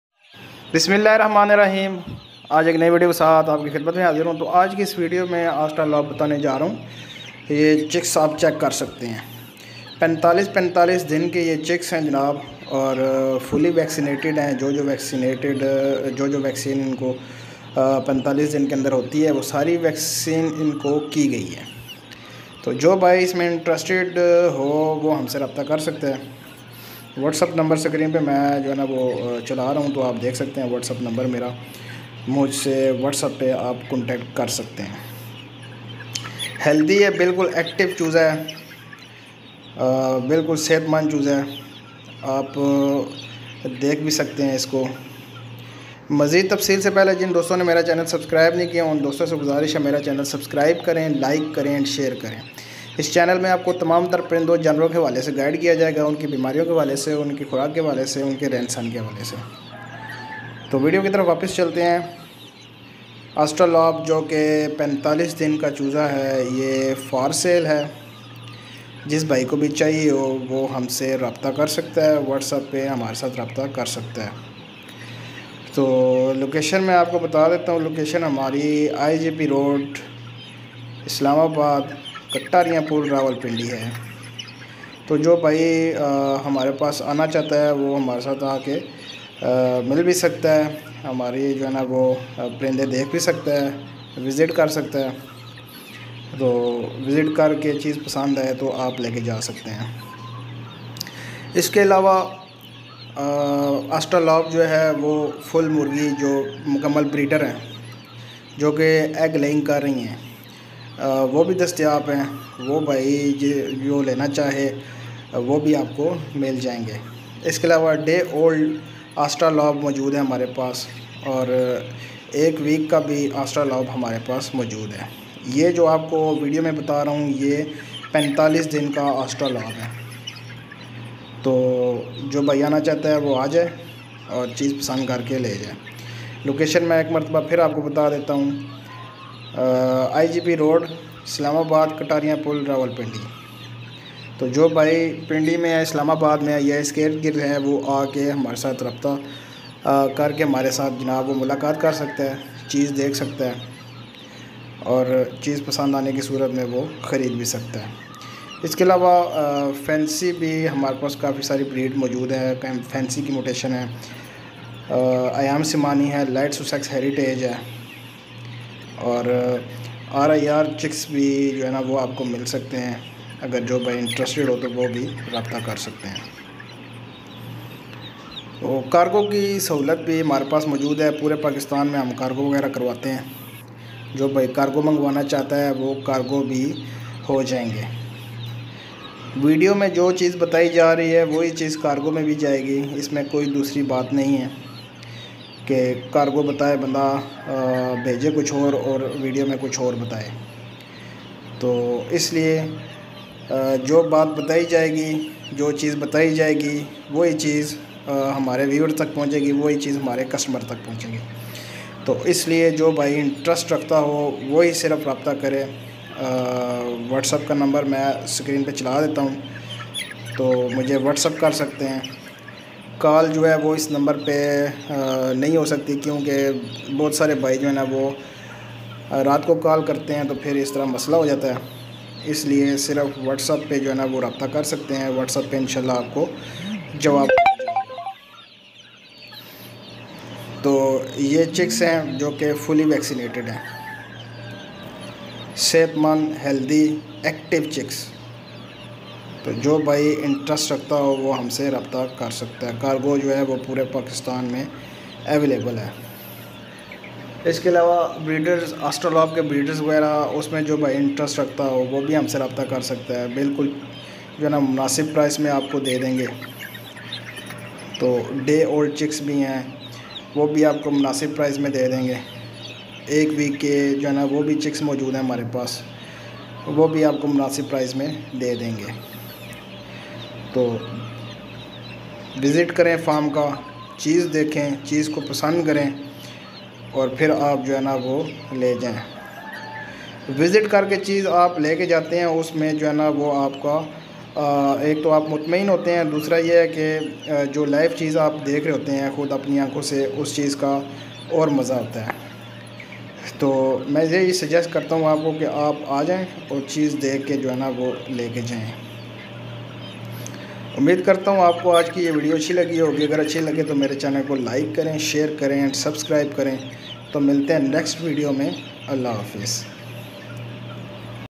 2 months old Australorp...2 month old chicks..2 mhine ka Australorp choza..
welcome to my YouTube chenal Pk Birds And Animals

Vedeo me 2 month old choza dikhane lga hon jo k ap bhaion k liay he jo bhai intrested hon WhatsApp pe rabta kr skte hn vedeo achi lge like share and subscribe krn thnx

#2 months old chicks

 WhatsApp num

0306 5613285

Riaz Naseem